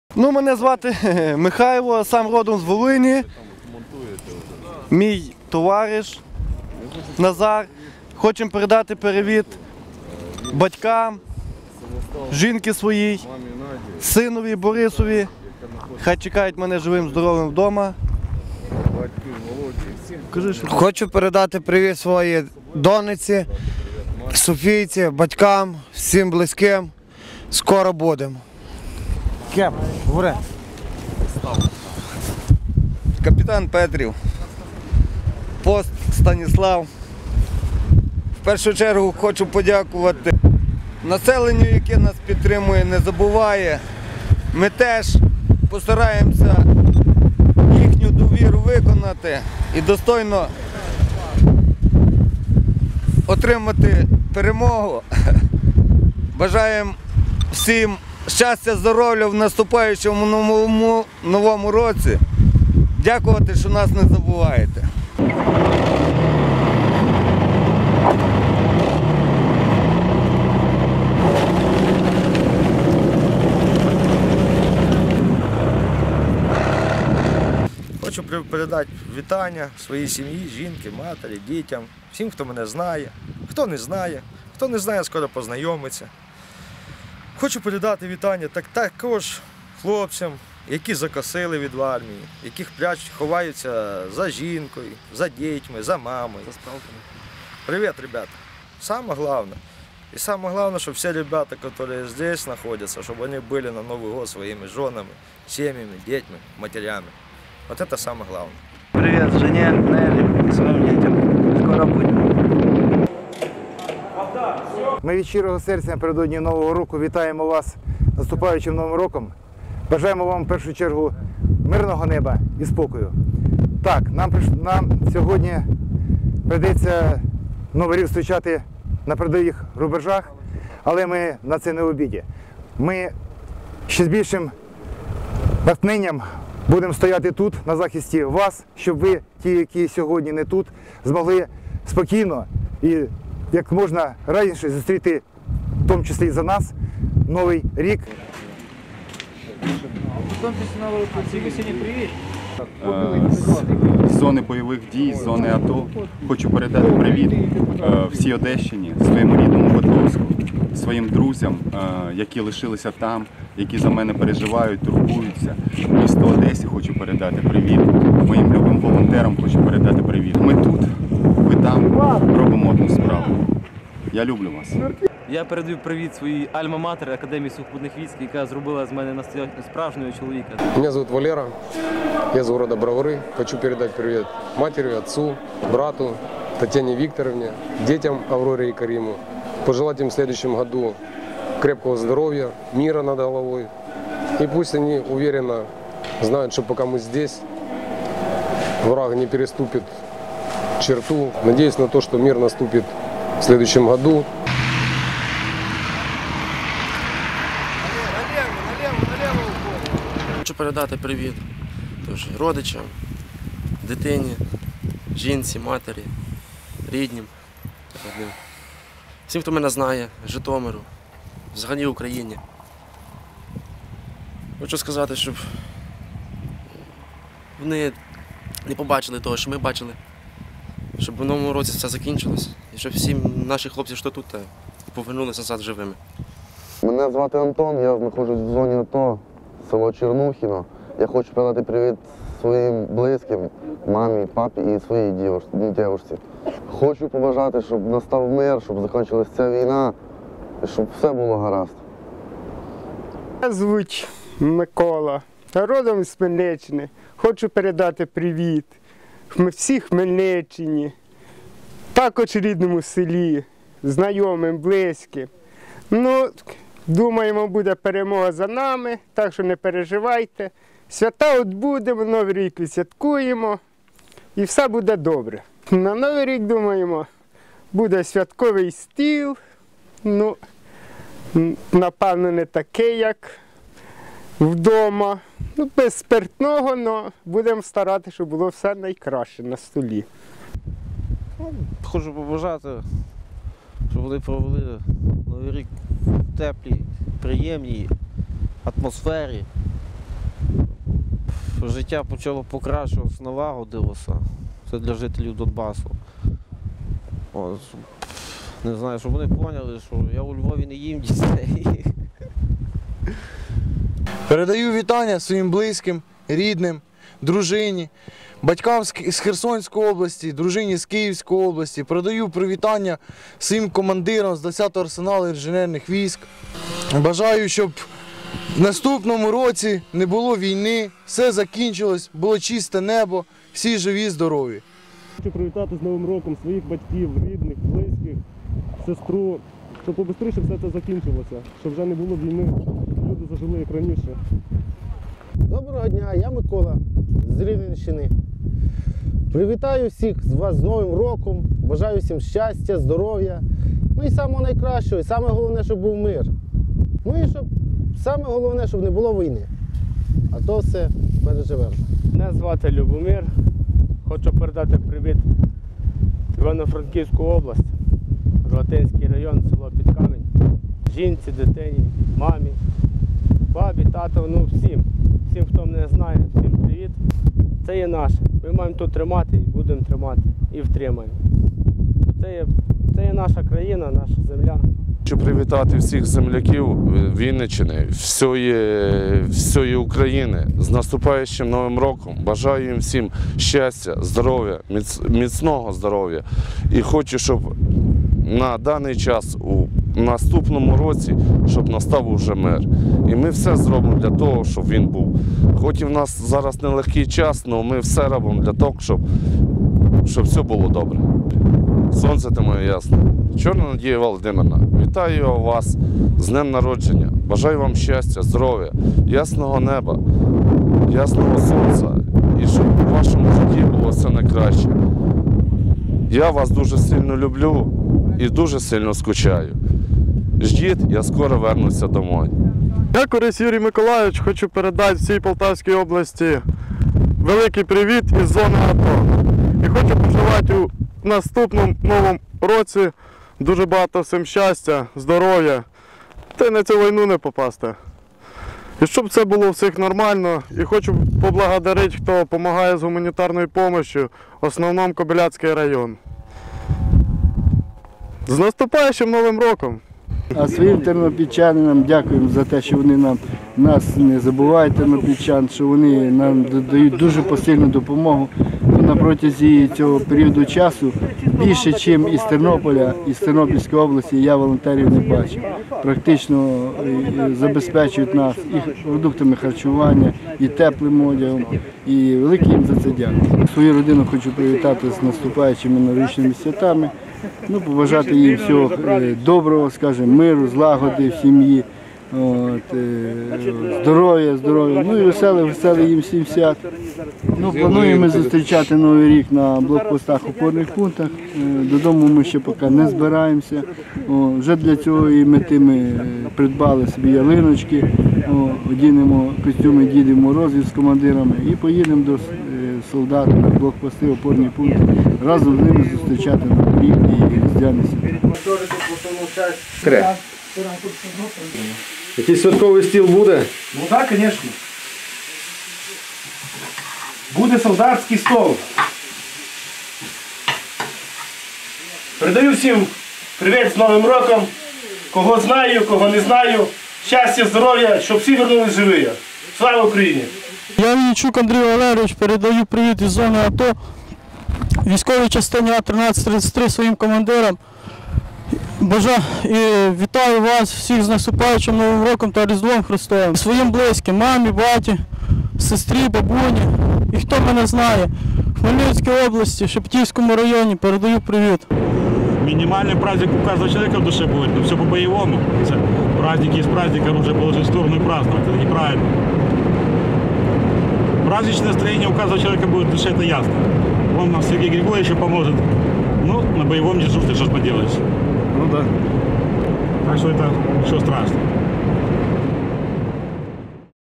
Щирі, яскраві, сердечні привітання від бійців з передової
Воїни, які стоять сьогодні на захисті нашої країни, - кіборги, водії бронетехніки, десантники, інженери, працівники тилу, хлопці, які несуть службу на передовій та блокпостах - передають вітання Україні, землякам, рідним, коханим та побратимам. Почуйте поздоровлення з дебальцівської передової, села Піски, з позицій під Лисичанськом та Горлівкою, а також інших місць, де несуть службу наші герої.